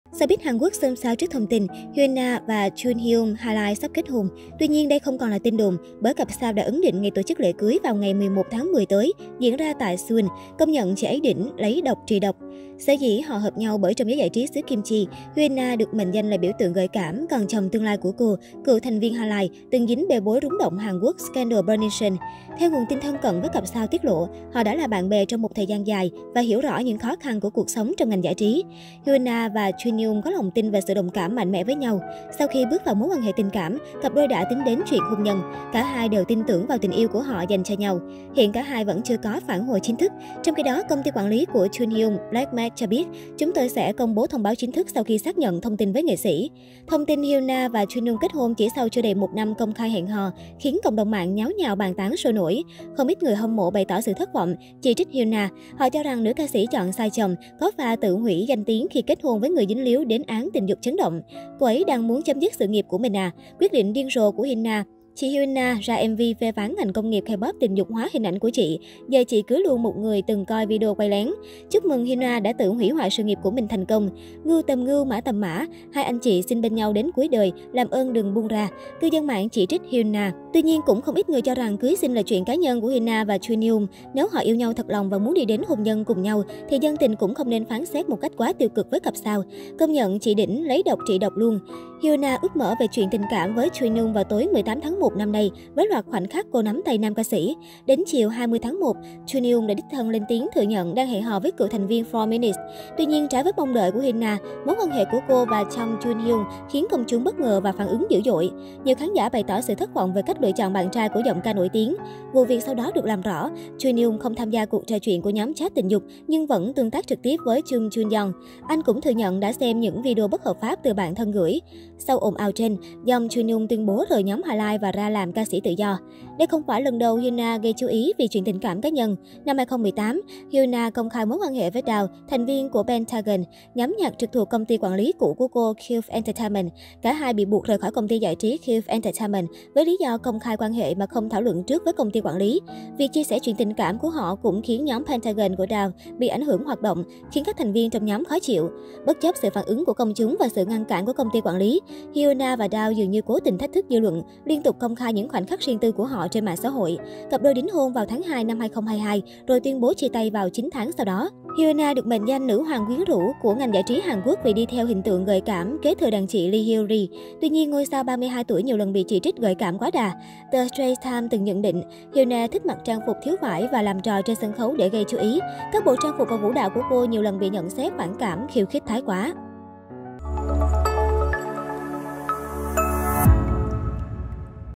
Hyuna chính thức được Junhyung chốt đơn
Showbiz Hàn Quốc xôn xao trước thông tin Hyuna và Junhyung (HIGHLIGHT) sắp kết hôn. Tuy nhiên, đây không còn là tin đồn, bởi cặp sao đã ấn định ngày tổ chức lễ cưới vào 11/10 tới, diễn ra tại Seoul, công nhận chị ấy đỉnh, lấy độc trị độc

Sở dĩ họ hợp nhau bởi trong giới giải trí xứ kim chi Hyuna được mệnh danh là biểu tượng gợi cảm, còn chồng tương lai của cô cựu thành viên Highlight từng dính bê bối rúng động Hàn Quốc - scandal Burning Sun. Theo nguồn tin thân cận với cặp sao tiết lộ “Họ đã là bạn bè trong 1 thời gian dài và hiểu rõ những khó khăn của cuộc sống trong ngành giải trí. Hyuna và Junhyung có lòng tin và sự đồng cảm mạnh mẽ với nhau. Sau khi bước vào mối quan hệ tình cảm, cặp đôi đã tính đến chuyện hôn nhân. Cả 2 đều tin tưởng vào tình yêu của họ dành cho nhau”.
Hiện, cả 2 vẫn chưa có phản hồi chính thức. Trong khi đó, công ty quản lý của Junhyung - Black Made - cho biết: "Chúng tôi sẽ công bố thông báo chính thức sau khi xác nhận thông tin với nghệ sĩ".